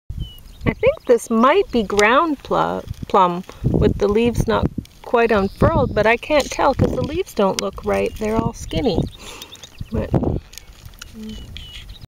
Mystery Pea Plant in Alexander Wilderness Nature Park.
May 14, 2023. 11:21 AM. Alexander Wilderness Nature Preserve, Lethbridge, Alberta, Canada.
I just went out for a walk with nothing but this book, my house keys, camera, and my flower songs. This book doesn't have many flowers but it is light (and easy to pack along on a walk).